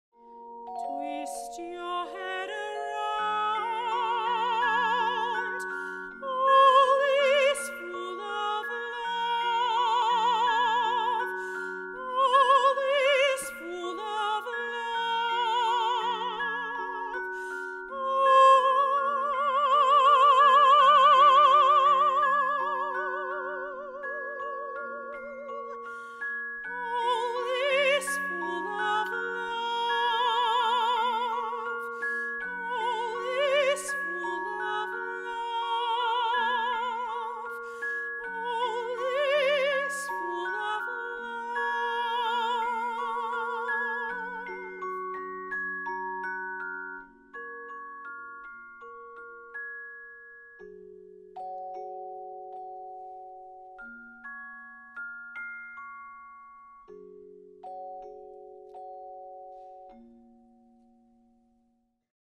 Björk Alternative Concert Trailer
On June 24th 2019 Milla Mihova (soprano) and Nikolay Stoykov (harpsichord, piano, celeste) will perform Björk Alternative at Oborishte 5 Gallery and Hall, Sofia.